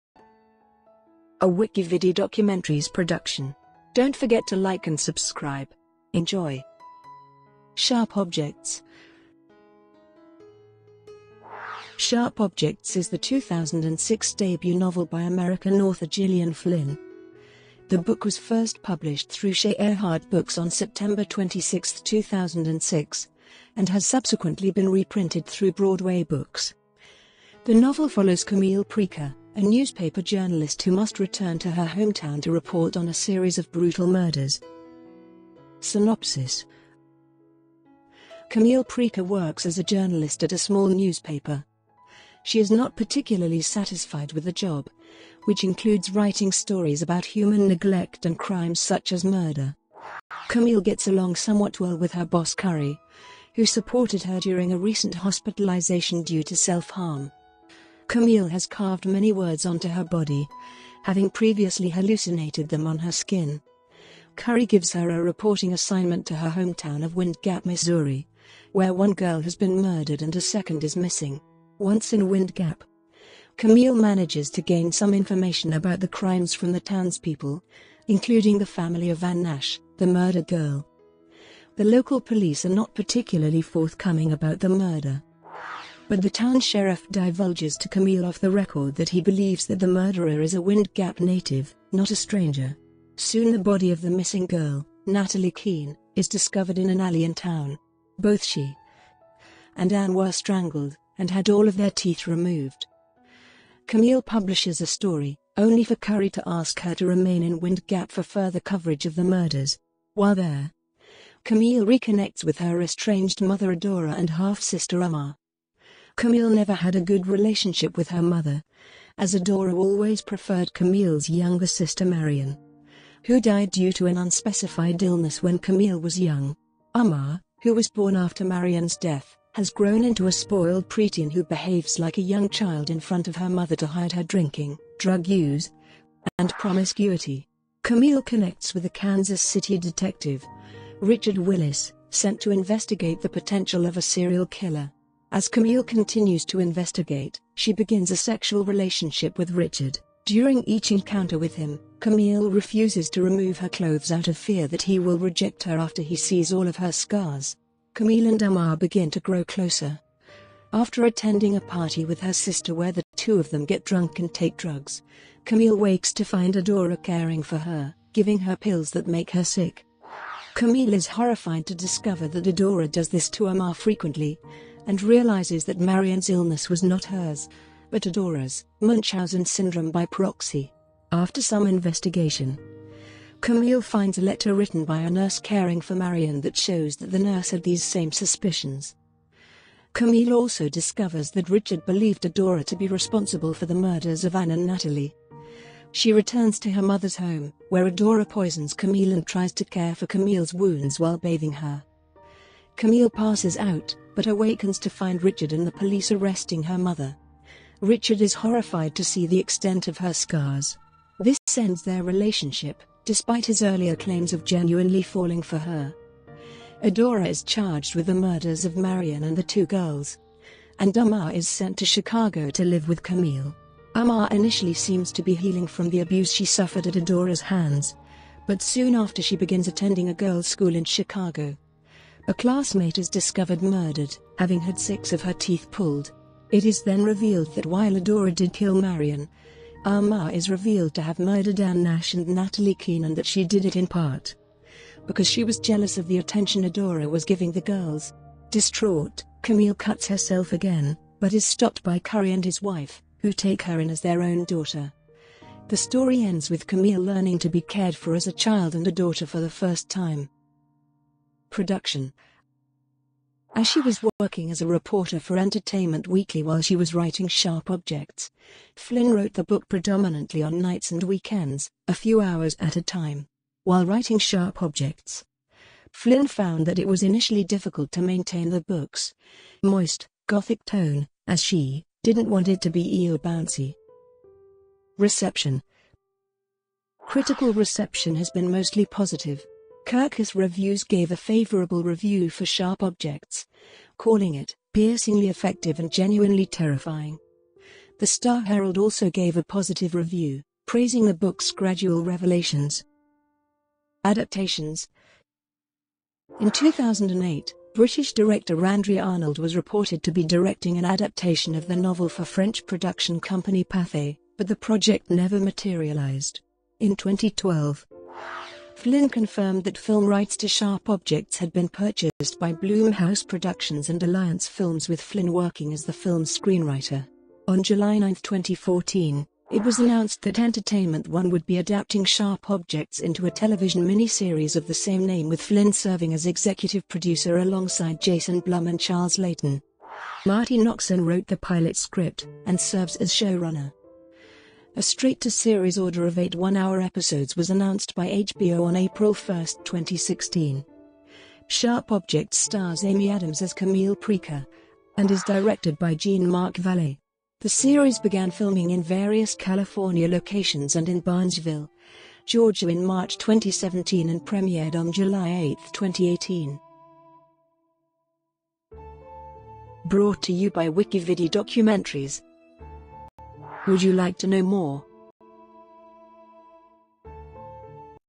SHARP OBJECTS - WikiVidi Documentary
Sharp Objects is the 2006 debut novel by American author Gillian Flynn. The book was first published through Shaye Areheart Books on September 26, 2006, and has subsequently been re-printed through Broadway Books. The novel follows Camille Preaker, a newspaper journalist who must return to her hometown to report on a series of brutal murders.

Shortcuts to chapters:
00:00:35 Synopsis
00:04:21 Production
00:04:44 Reception

Copyright WikiVidi.